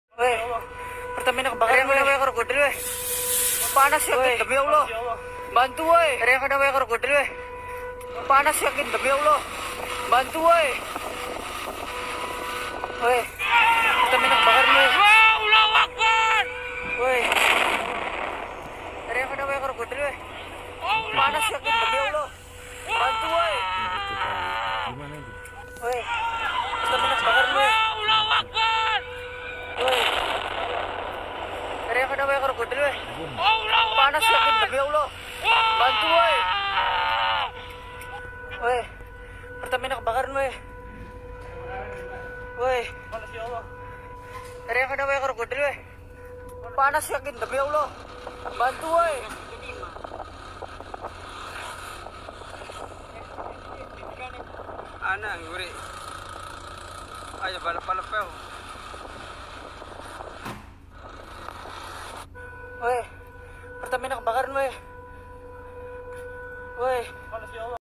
Kilang minyak Pertamina Balongan Indramayu meledak kebakaran II
Kilang minyak PT Pertamina RU VI Balongan, Kabupaten Indramayu, Jawa Barat kebakaran. Kabid Damkar Indramayu, Joni Takarasel mengatakan kebakaran mulai terjadi sekitar pukul 00.30 WIB.
"(Kebakaran) jam 12 lebih, setengah 1 lah," ujar Joni saat dihubungi, Senin (29/3/2021).